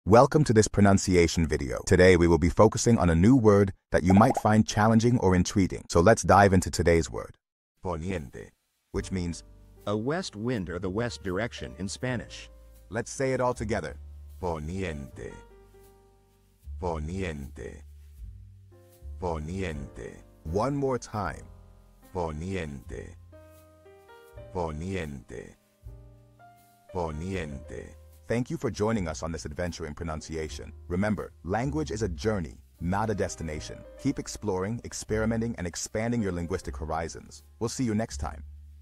How to pronounce Poniente
Master the Pronunciation of 'Poniente Which Means Poniente' - which means : To West Wind or The West Direction in Spanish. 📝 with @PronunciationSpanish 🎓🗣️ - [Your Guide to Spanish]

Learn to pronounce 'Poniente' perfectly in Spanish with our easy and engaging tutorial by @PronunciationSpanish! 📖📽️ In this comprehensive video, we provide you with native speaker examples 🗣️👂 and phonetic breakdowns 🎵 to ensure you get the pronunciation just right.

How to say West in Spanish